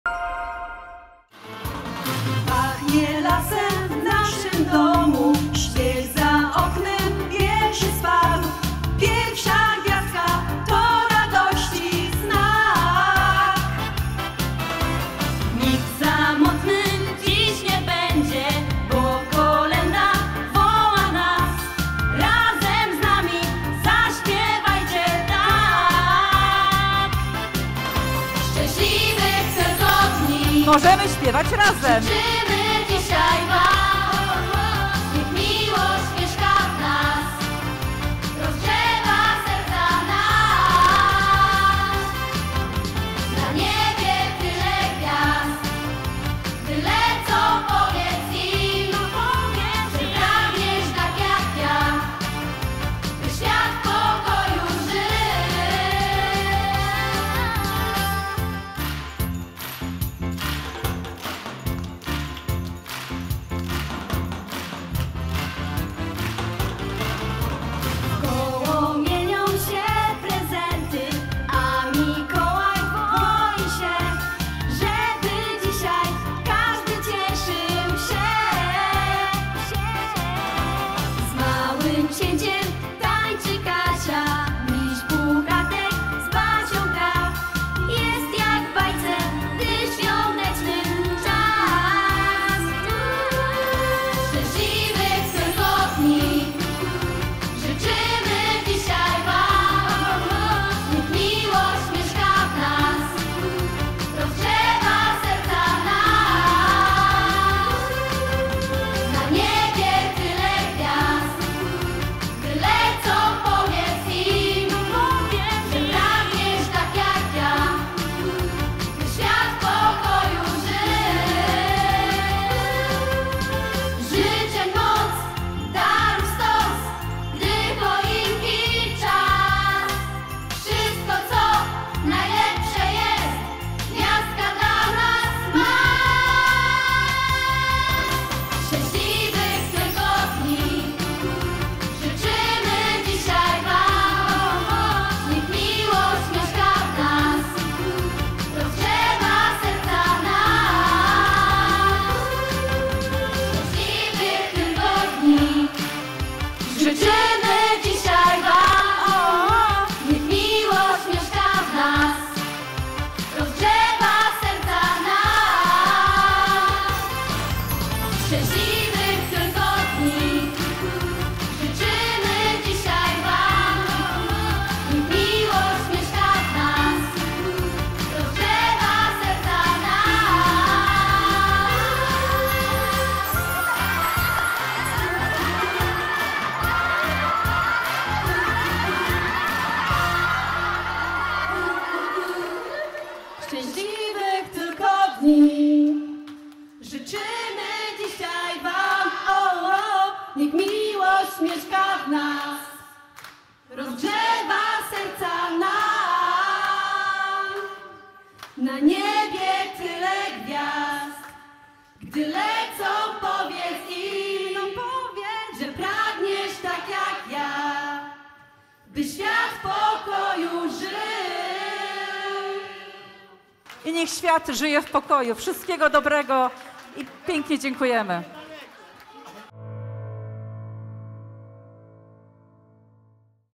Koncert świąteczny OKiT Barwice - finał
Zapraszamy na fragment świątecznego koncertu podopiecznych i przyjaciół Ośrodka Kultury i Turystyki w Barwicach, który dla Państwa zarejestrowaliśmy w lokalnej Sali Miejskiej.